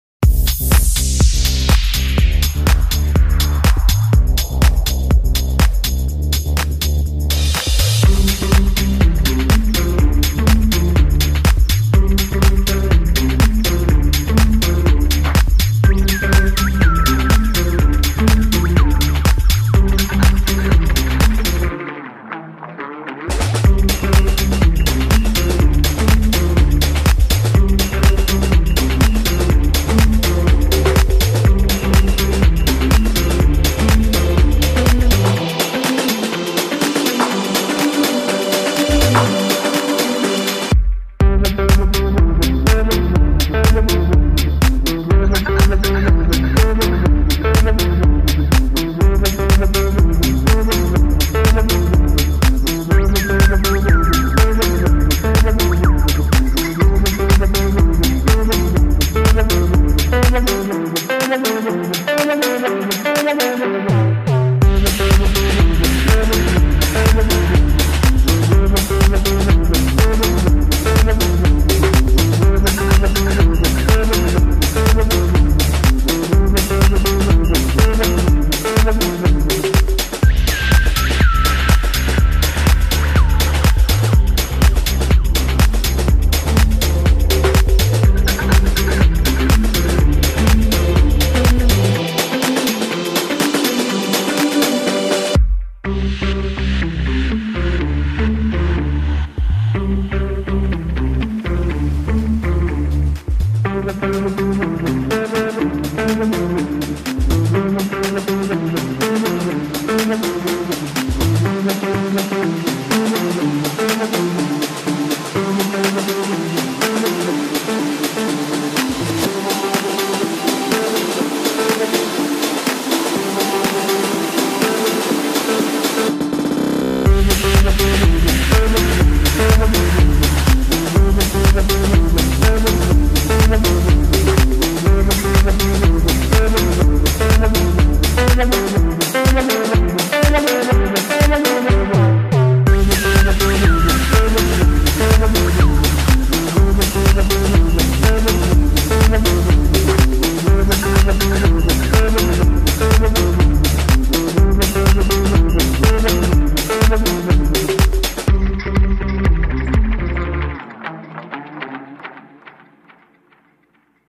20 driepunters van BS Weert uit de periode 1998-2000
20 driepunters van BS Weert uit de periode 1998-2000 met onder meer Ralph Biggs, Kevin Thompson en Gerard Ackermans.